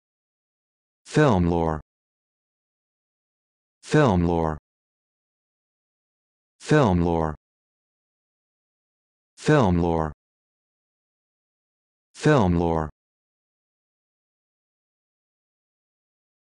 How to Pronounce Filmlore
This video shows you how to pronounce Filmlore